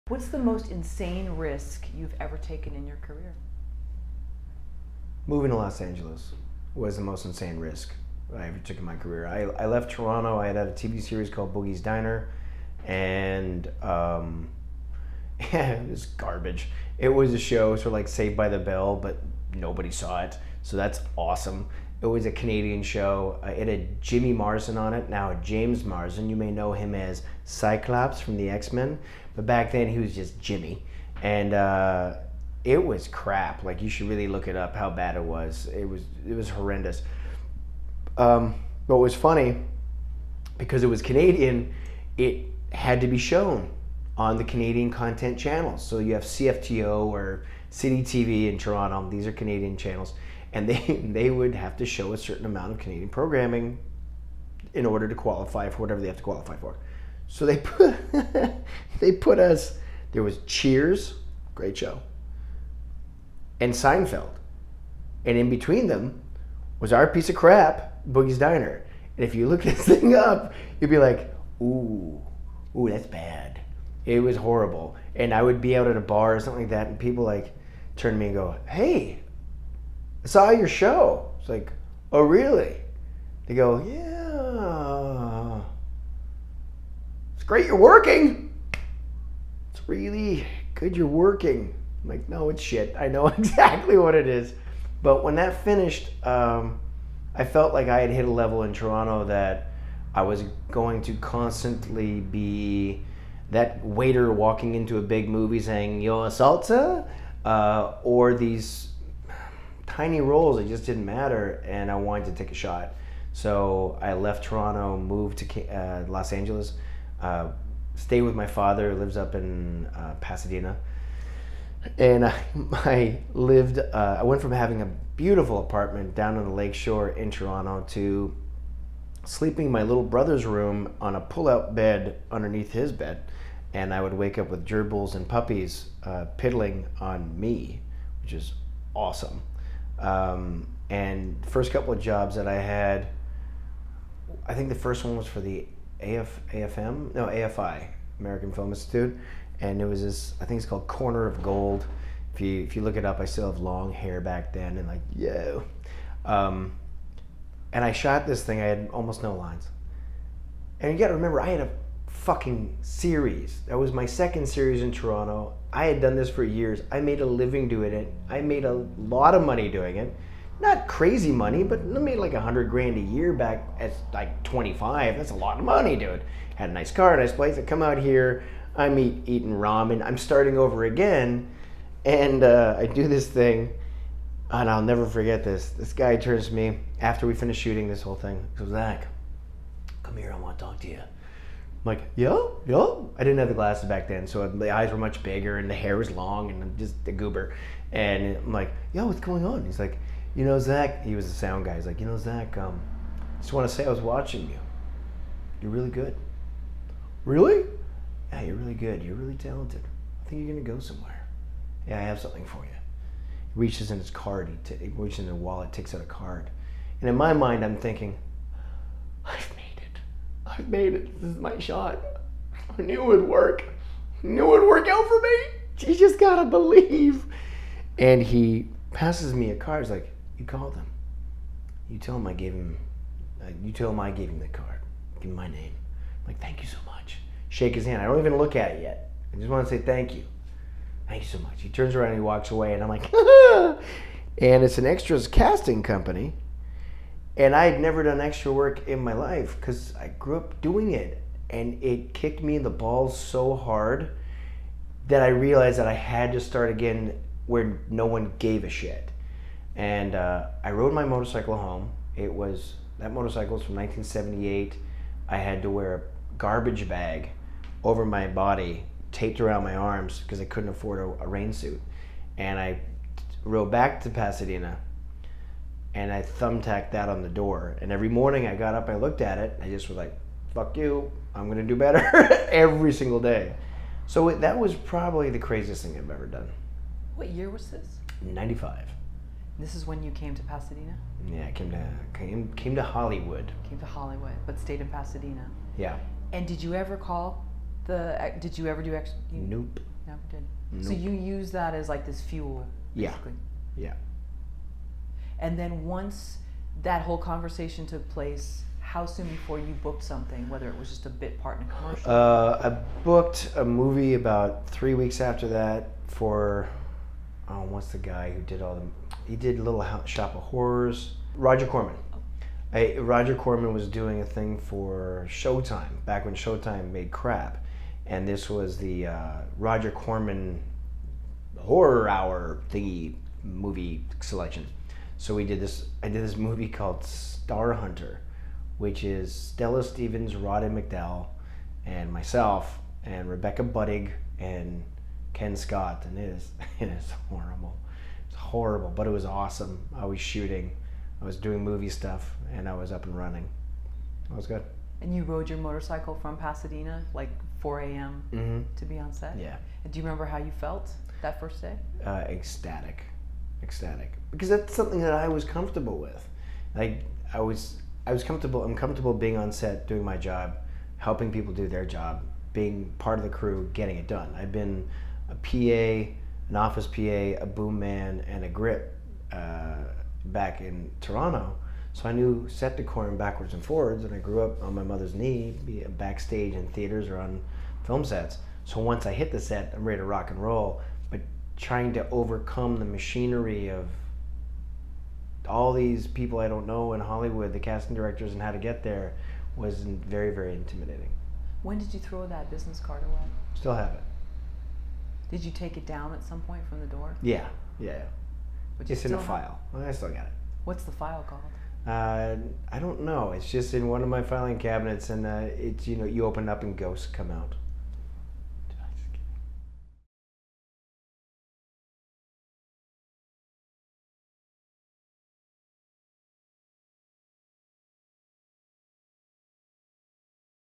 Zack Ward On How A Business Card Fueled His Acting Career
MORE VIDEOS WITH ZACK WARD